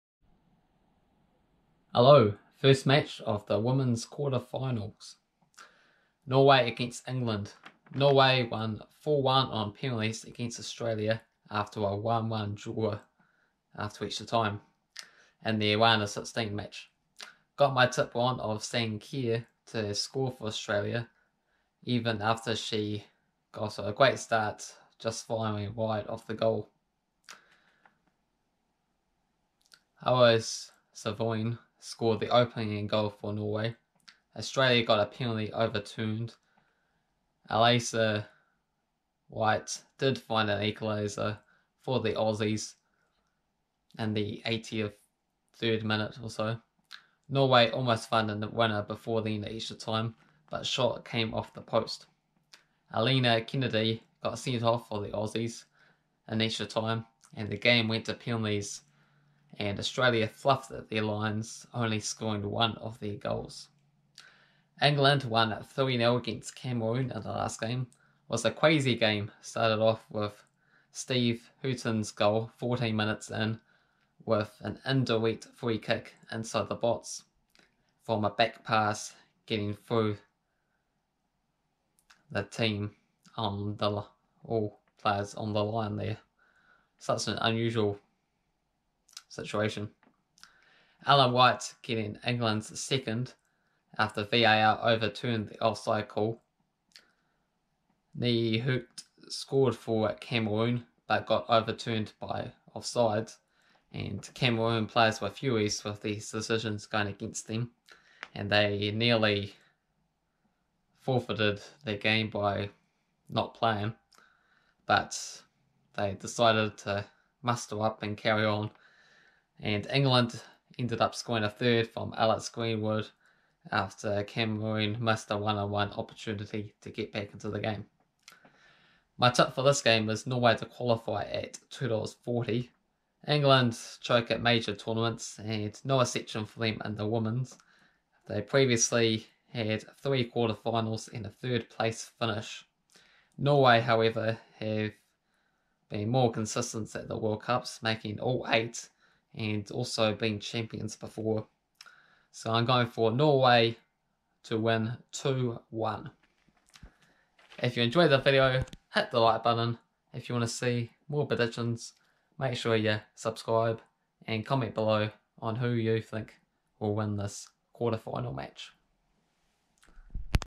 Women's football world cup 2019 Norway vs England quarterfinal
preview of my prediction of upcoming women's football 2019 world cup
match between Norway and England
Tip Norway to qualify 2.40
score 2-1 Norway